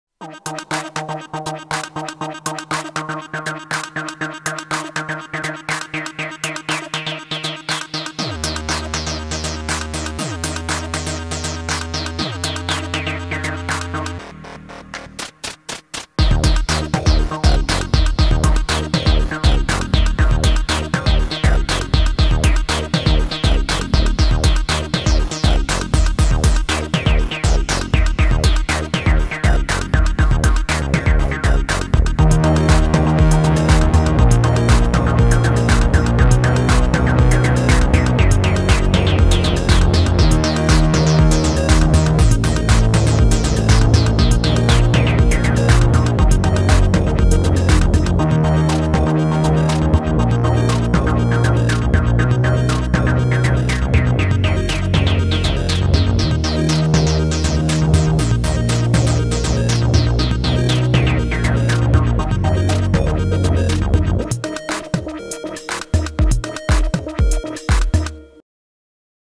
Techno with Nintendo switch feat. Subsequent 25
First project in Korg Gadget I filmed featuring Moog on the Bass.